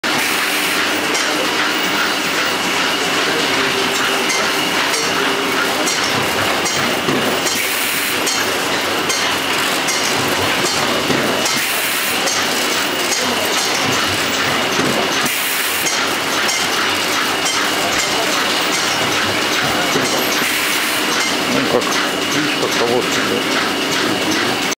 Так лечат простуду
Конвейер бежит ,  упаковки  кагоцела слетают. Обнинск.